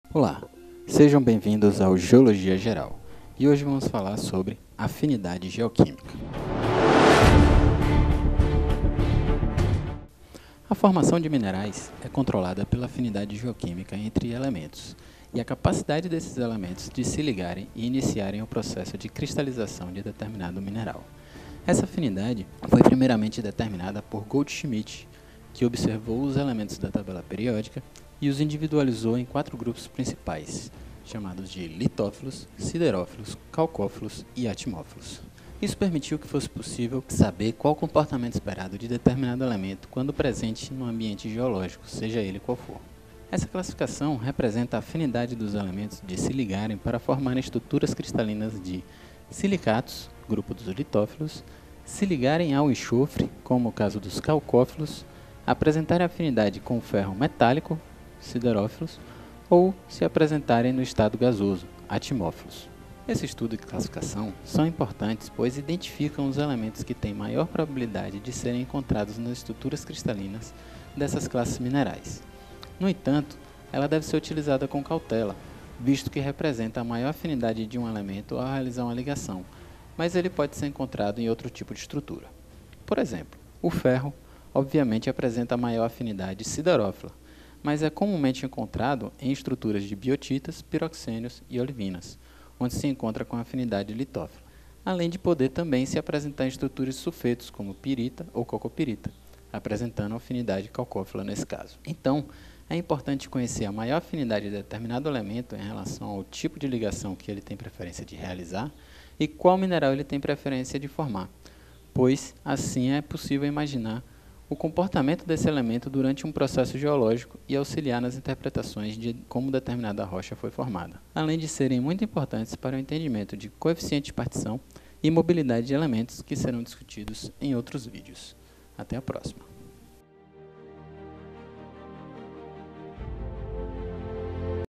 Afinidade Geoquímica - Classificação de Goldschmidt
*Errata 1:30 Fórmula da Olivina (Mg,Fe)2SiO4

Um pouco sobre classificação de Goldschmidt e afinidade geoquímica de elementos para formação de minerais.

"A formação de minerais é controlada pela afinidade geoquímica entre elementos e a capacidade desses elementos de se ligarem e iniciarem o processo de cristalização de determinado mineral.

Essa afinidade foi primeiramente determinada por Goldschmidt, que observou os elementos tabela periódica e os individualizou em 4 grupos principais: litófilos, siderófilos, calcófilos e atmófilos. Isso permitiu q fosse possível saber qual seria o comportamento esperado de determinado elemento quando presente num ambiente geológico seja ele qual for.

Essa classificação representa a afinidade dos elementos de se ligarem para formarem estruturas cristalinas de silicatos (grupo dos litófilos), se ligarem ao enxofre (como os calcófilos), apresentarem afinidade com o ferro metálico (siderófilos) ou se apresentarem no estado gasoso (atmófilos).

Esse estudo e classificação são importantes, pois identificam os elementos que tem maior probabilidade de serem encontrados nas estruturas cristalinas dessas classes minerais. No entanto, ela deve ser utilizada com cautela, visto que representa a maior afinidade de um elemento ao realizar uma ligação, mas ele pode ser encontrado em outro tipo de estrutura.

Por exemplo, o Fe, obviamente apresenta maior afinidade siderófila, mas é comumente encontrado em estruturas de biotitas, piroxênios e olivinas, onde se encontra com afinidade litófila. Além de poder também se apresentar em estruturas de sulfetos como pirita ou calcopirita, apresentando afinidade calcófila nesse caso.

Então, é importante conhecer a maior afinidade de determinado elemento em relação ao tipo de ligação que ele tem preferência de realizar e qual mineral ele tem preferência de formar, pois assim é possível imaginar o comportamento deste elemento durante um processo geológico e auxiliar nas interpretações de como determinada rocha foi formada, além de serem muito importantes para o entendimento de coeficiente de partição e mobilidade de elementos."